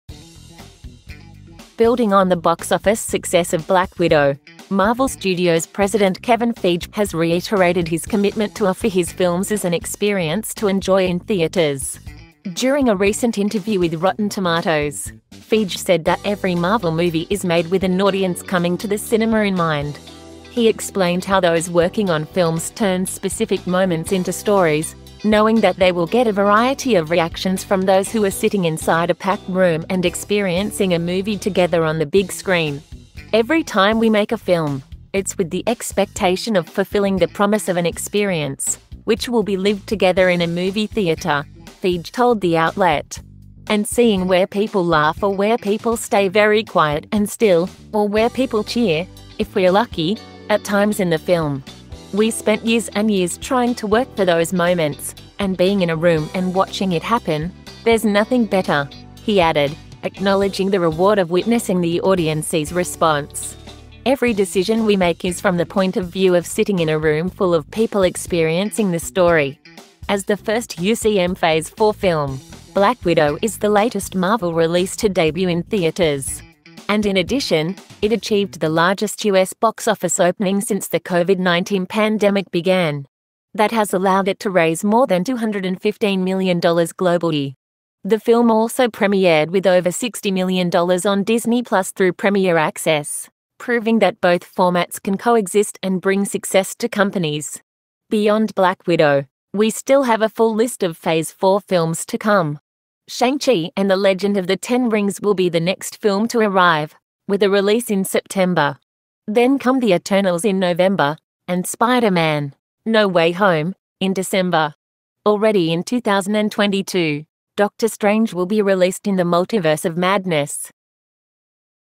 Kevin Feige builds on the success of Black Widow in theaters and talks about how to make Marvel film
Kevin Feige builds on the success of Black Widow in theaters and talks about how to make Marvel movies
Marvel has reconfirmed that the UCM films are meant to fill movie theaters. So, every decision that is made, it is in that sense